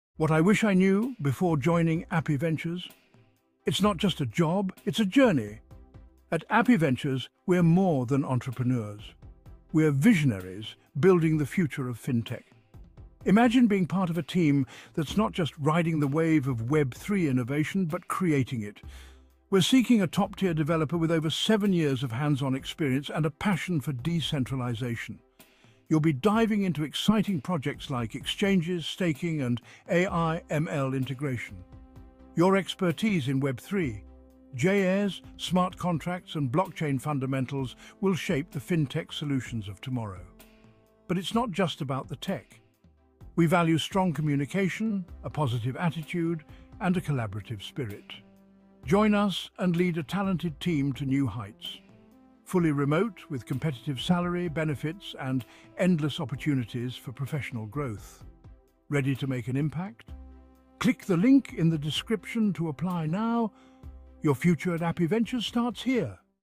Remote Web3 developer job at Appy Ventures | Open to people anywhere in the world
Application Process:

Stage 1: Interested candidates, please submit details in the application form

Stage 2 will be an asynchronous video interview (because no one sensible enjoys too many meetings)

Stage 3 will be the Tech test

Stage 4 will be the Interview with our CTO and CEO